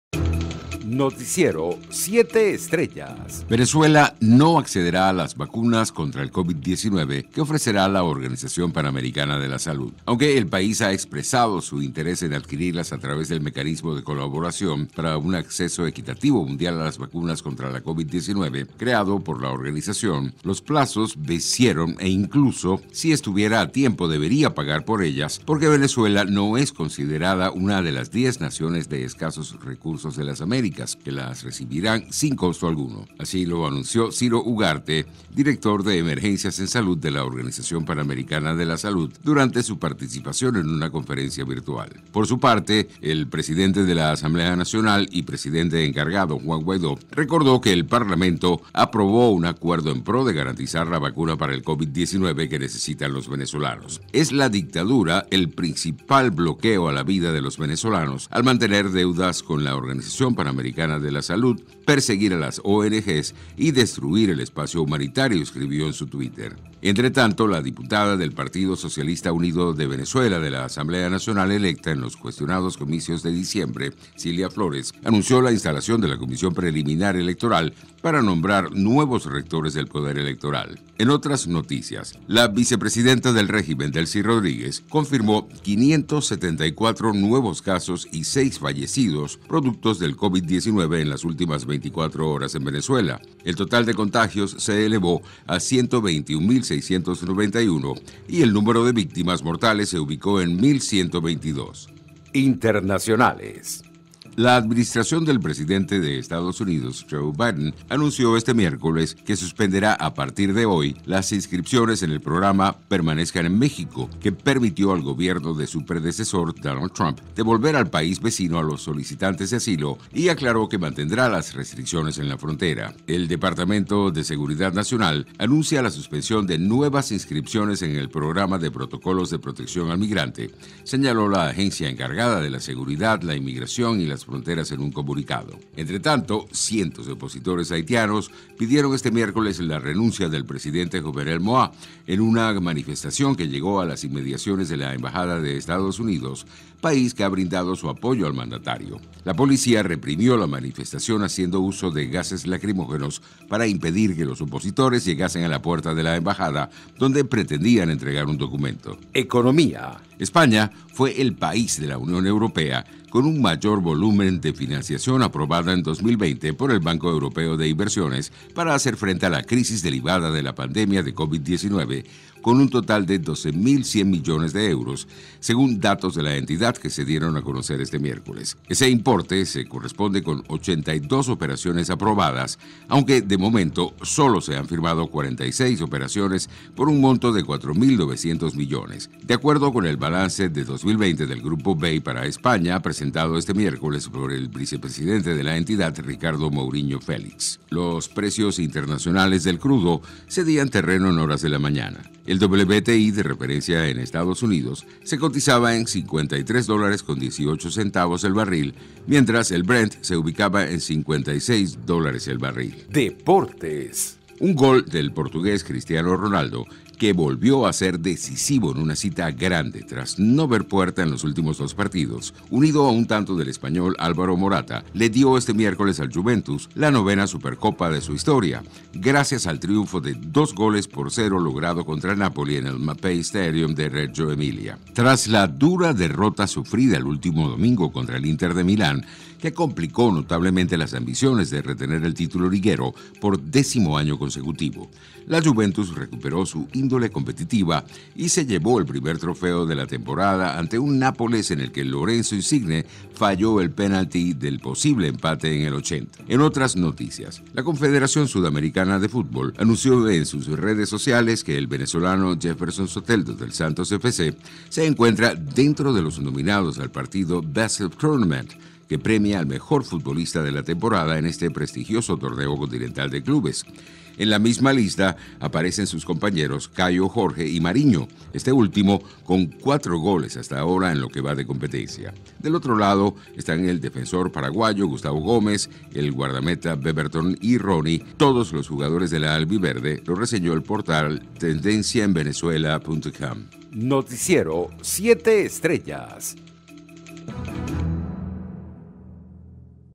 Venezuela no accederá a las vacunas que ofrecerá la Organización Panamericana de la Salud
🌡Venezuela no accederá a las vacunas contra el COVID-19 que ofrecerá la Organización Panamericana de la Salud

🌡El Presidente de la Asamblea Nacional y Presidente encargado, Juan Guaidó recordó que el Parlamento aprobó un acuerdo en pro de garantizar la vacuna para el Covid-19 que necesitan los venezolanos

“Es la dictadura el principal bloqueo a la vida de los venezolanos al mantener deuda con la OPS, perseguir a las ONGs y destruir el espacio humanitario”, escribió en su Twitter

📌La Diputada del Partido Socialista Unido de Venezuela de la Asamblea Nacional electa en los cuestionados comicios de diciembre, Cilia Flores anunció la instalación de la Comisión Preliminar Electoral para nombrar nuevos rectores del Poder Electoral

🌡La vicepresidenta del régimen, Delcy Rodríguez confirmó 574 nuevos casos y seis fallecidos producto del COVID-19 en las últimas 24 horas en Venezuela

En total de contagios se elevó a 121.691 y el número de víctimas mortales se ubicó en 1.122

INTERNACIONALES

📌La Administración del Presidente de Estados Unidos, Joe Biden anunció este miércoles que suspenderá a partir de mañana las inscripciones en el programa "Permanezcan en México", que permitió al Gobierno de su predecesor, Donald Trump, devolver al país vecino a los solicitantes de asilo, y aclaró que mantendrá las restricciones en la frontera

🗣Cientos de opositores haitianos pidieron este miércoles la renuncia del presidente Jovenel Moise en una manifestación que llegó a las inmediaciones de la embajada de Estados Unidos

ECONOMÍA

🌡España fue el país de la Unión Europea con un mayor volumen de financiación aprobada en 2020 por el Banco Europeo de Inversiones (BEI) para hacer frente a la crisis derivada de la pandemia de COVID-19

🛢Los precios internacionales del crudo cedían terreno en horas de la mañana

El WTI de referencia en Estados Unidos se cotizaba en 53.18 dólares el barril, mientras el Brent se ubicaba 56 dólares el barril

DEPORTES

⚽Un gol del portugués Cristiano Ronaldo, unido a un tanto del español Álvaro Morata, le dio este miércoles al Juventus la novena Supercopa de su historia